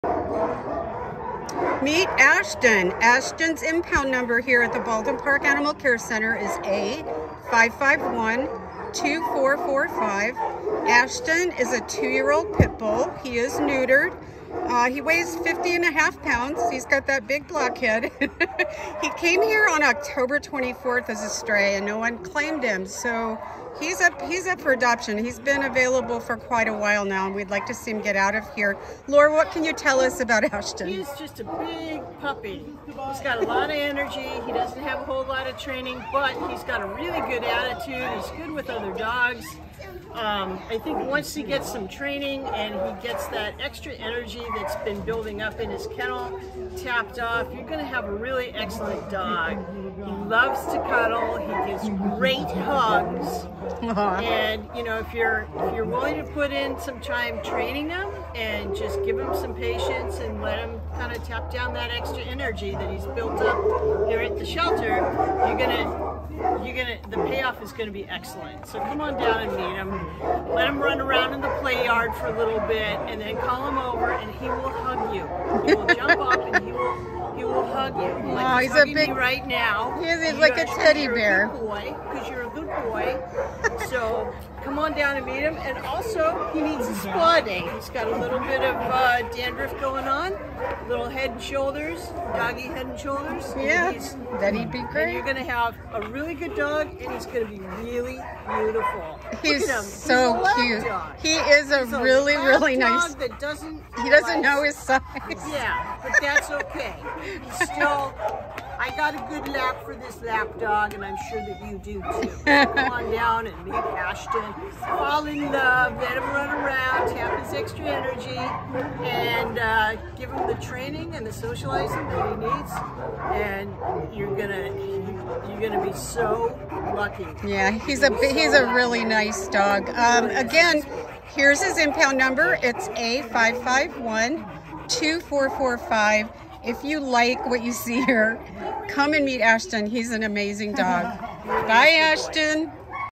A5512445 Ashton | Pit Bull
A5512445 Ashton is an affectionate 2 year old black with white neutered male pit bull who came to the Baldwin Park Animal Care Center on October 24 after being found as a stray. Weighing 51 lbs, Ashton loves snuggles and cuddles and will happily hug new people he meets. Young Ashton shows signs of housebreaking and needs all the puppy basic training including walking on leash. With a medium energy level, he will be a great walking, running or hiking buddy. He’s very interested in other dogs and friendly with them and would enjoy a large dog sibling. For now the shelter is requiring that any children in the home be at least 8 years to be the best playmates for him. A cute boy with bat like ears and a wrinkly snout, Ashton is ready to be someone’s special pal.